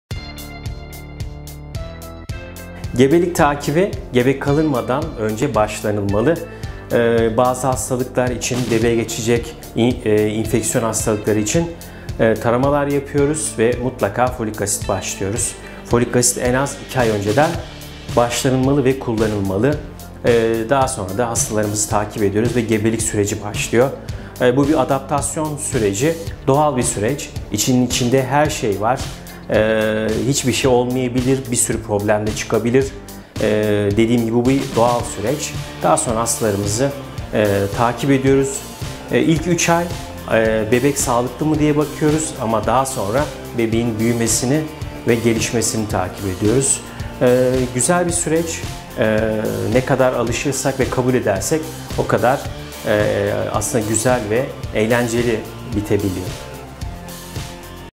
Gebelik süreci
Gebelik takibi, gebe kalmadan başlamalıdır. Riskler belirlenmeli ve tedavi edilmelidir. Takiplerinizi yaptırın...